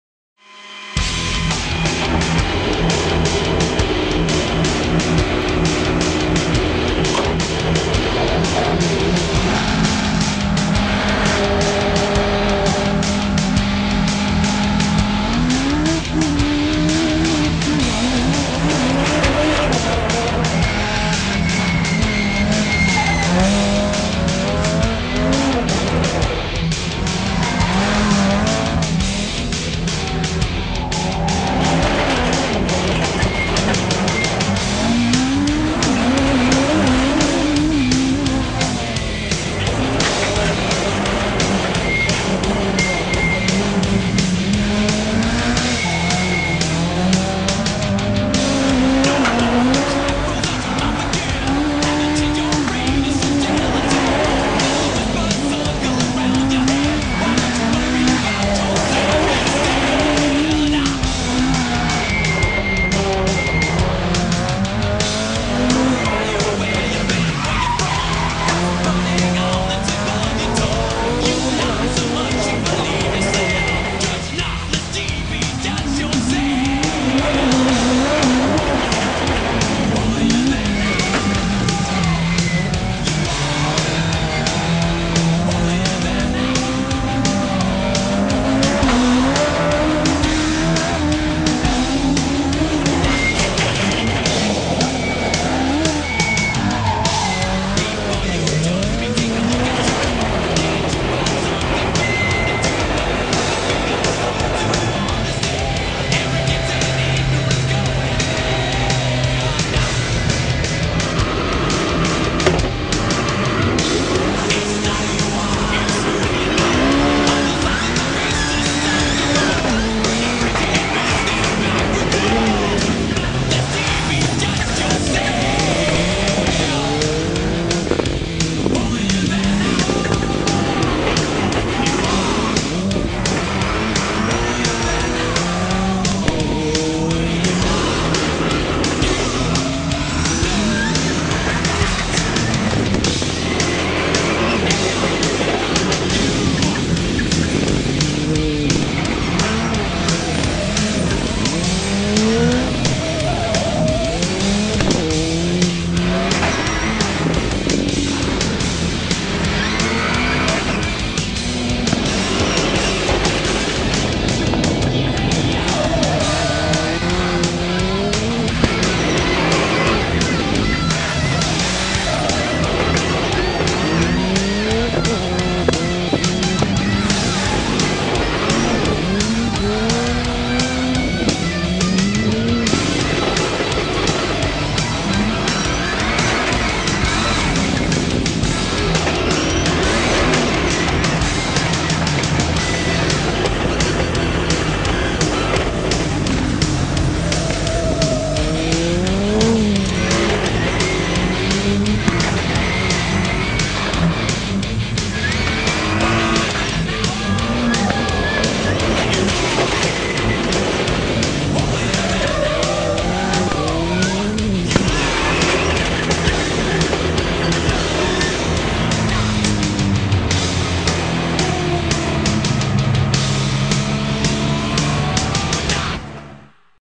This is Rallycross!!! The ultimativ ALL-INKL.COM RX Racing Tracktest
This is the ultimativ Testtrackvideo of the ALL-INKL.COM Muennich Motorsport RX Racing Team recorded on Schluechtern (Germany) Ewald-Pauli-Circuit.
René Muennich, current European Rallycross Championship Driver, tests the selfdeveloped Skoda Fabia MK1 S1600 and his Division1 Supercar Skoda Fabia MK1 T16 4x4 with nearly 600HP at the brandnew German Rallycross Championship Track.